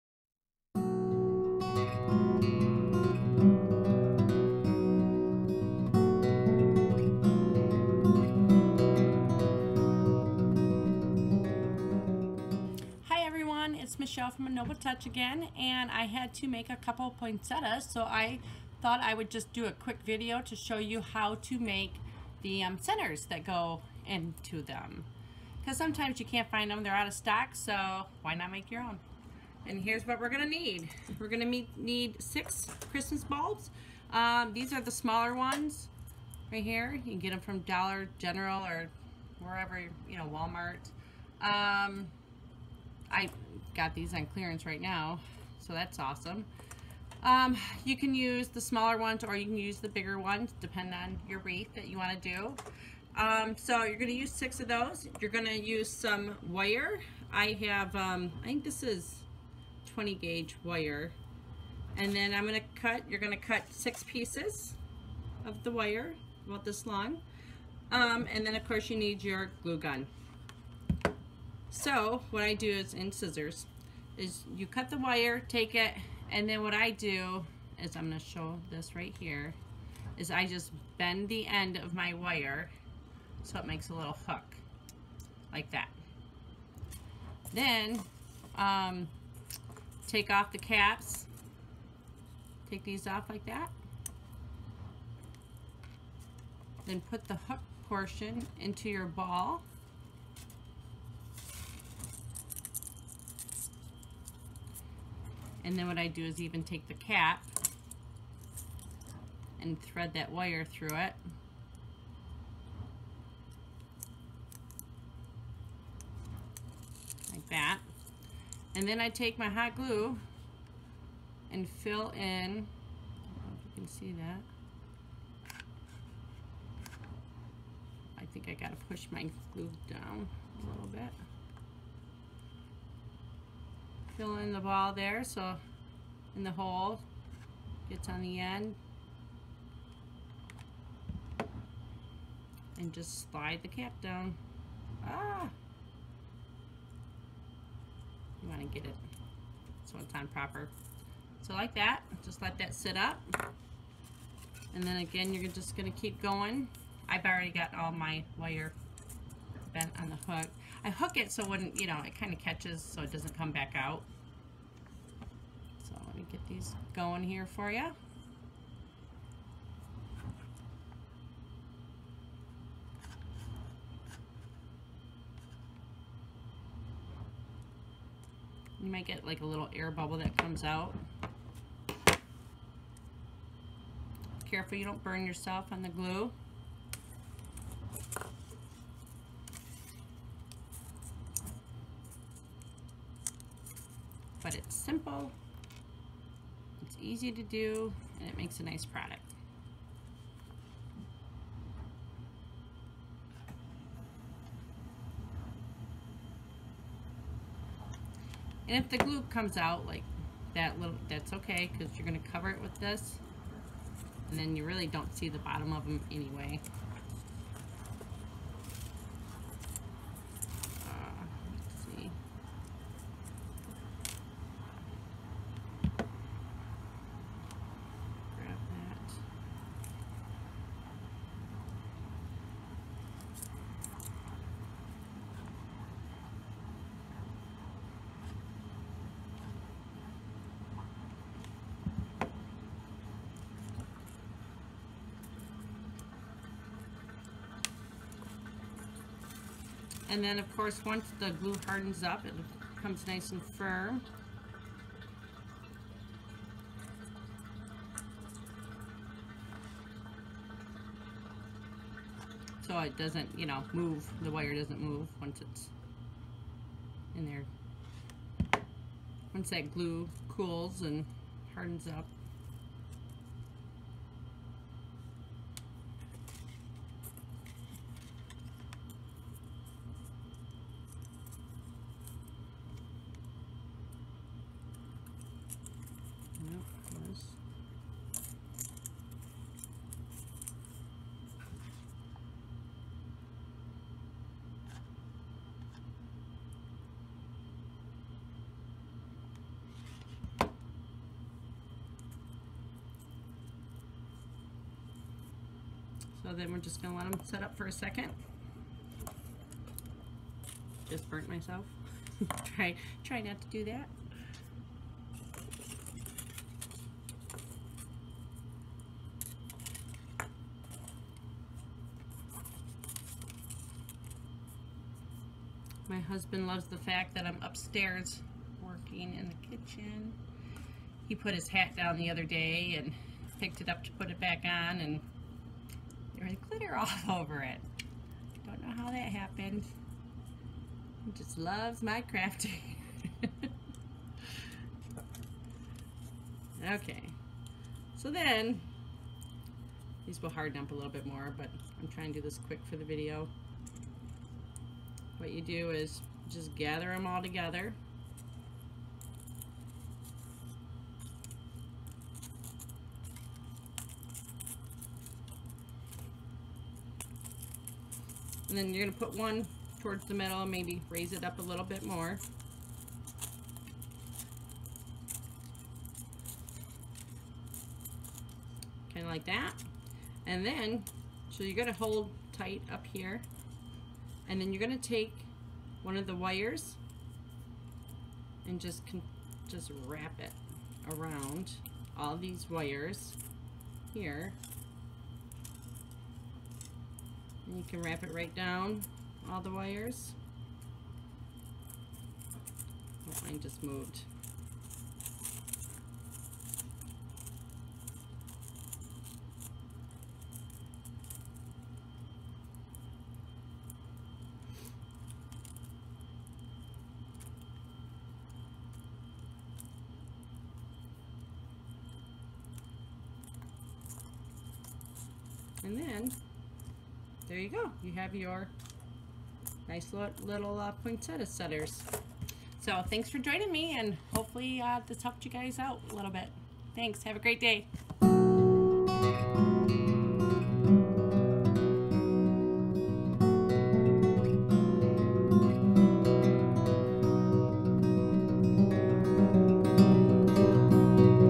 DIY Poinsettia Center for your Flower
Michelle walks you through making a poinsettia center for your flower wreath.